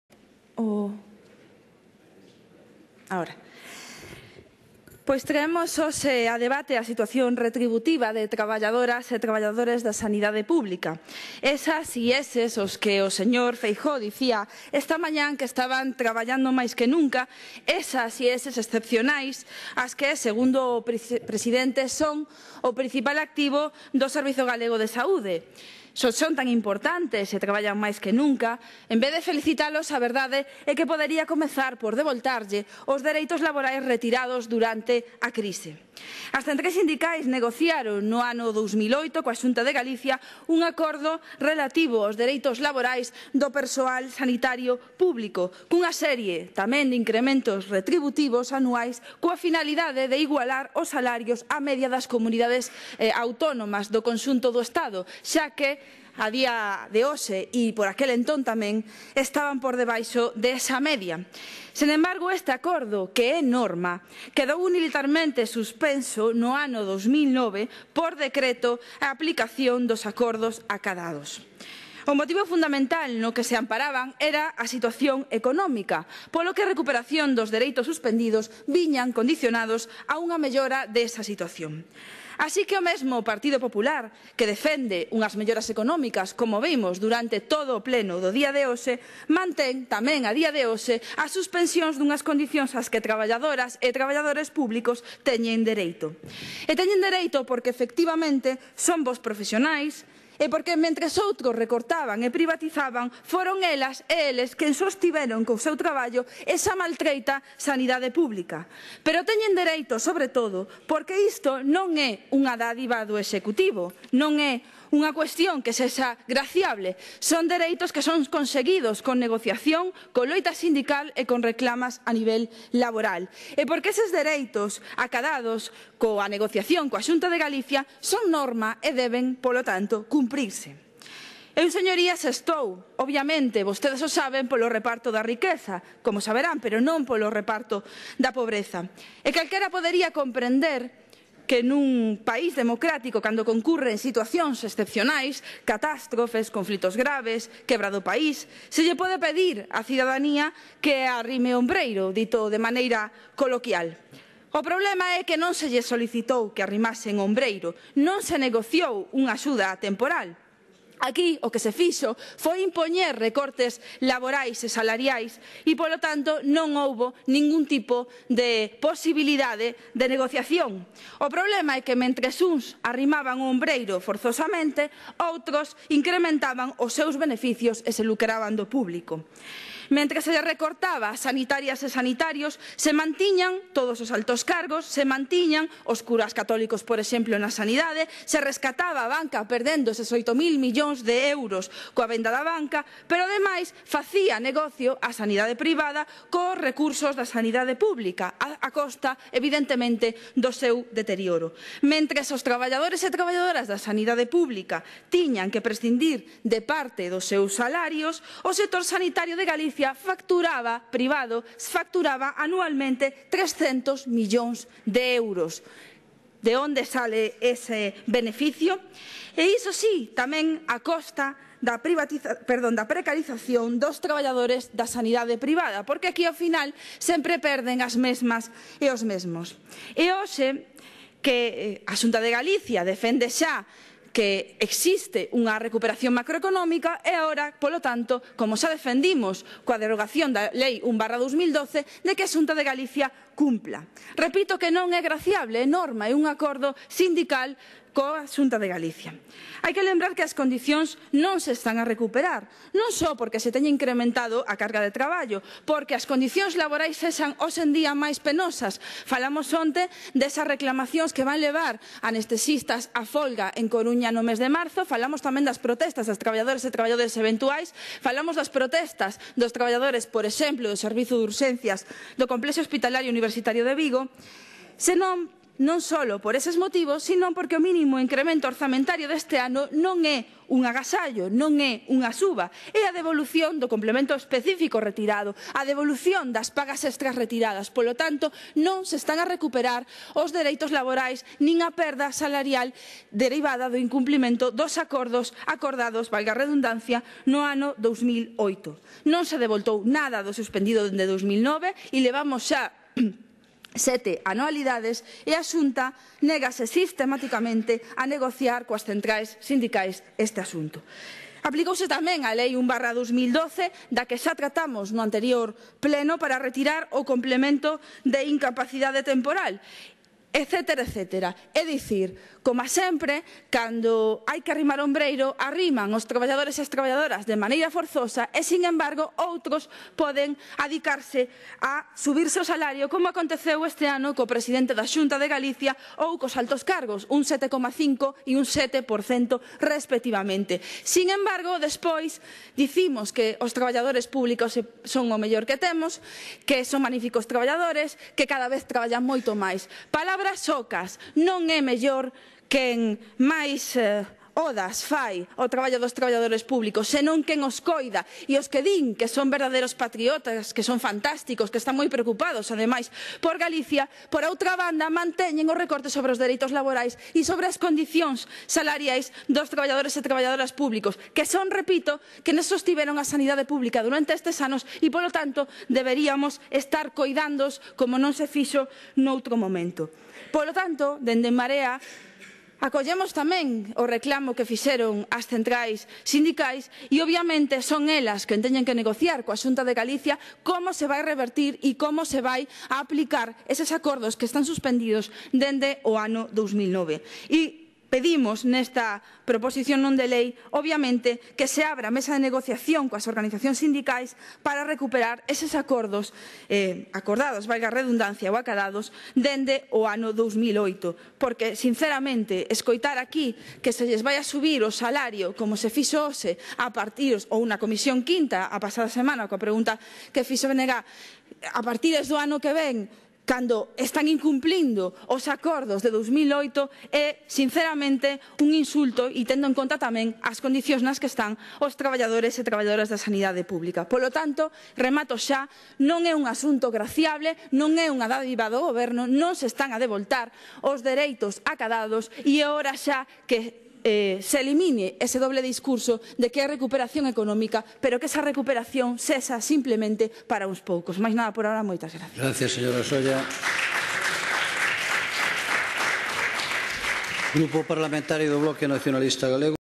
Eva Solla esixe á Xunta que se sente a negociar co persoal do SERGAS
Na pasada sesión plenaria, Eva Solla, membro da dirección de Esquerda Unida e vicepresidenta segunda do Parlamento de Galicia por En Marea, defendeu unha proposta que recollía as demandas sindicais para solicitar á Xunta que se sentara a negociar coas e cos representantes sindicais a devolución dos dereitos retributivos e laborais do persoal do SERGAS.

O PP votou en contra dunha proposta de mínimos que esixía dita negociación.